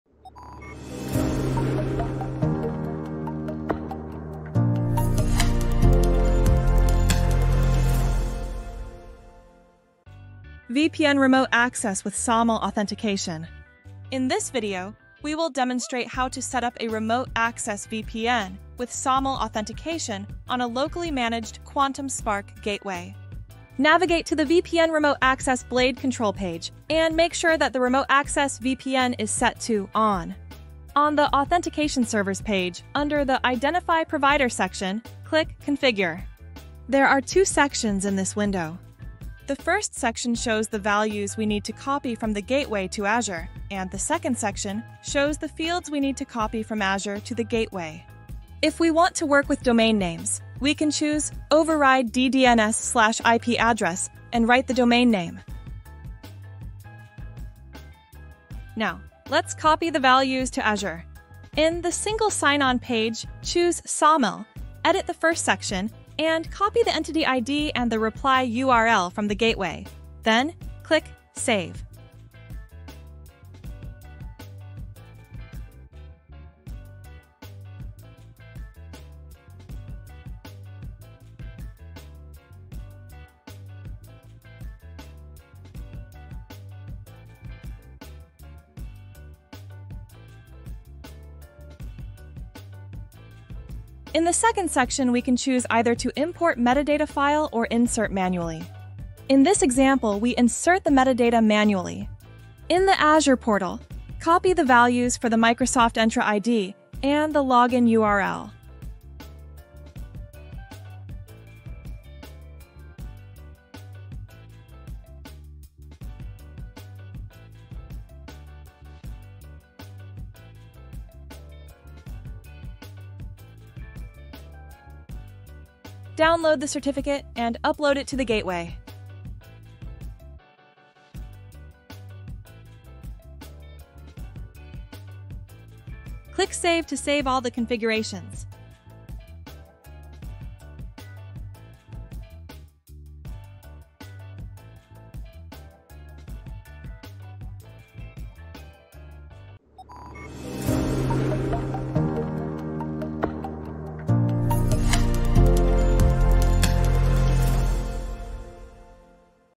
Check Point Quantum Spark: VPN Remote Access with SAML Authentication
Learn how to set up a remote access VPN with SAML authentication on a locally managed Quantum Spark gateway.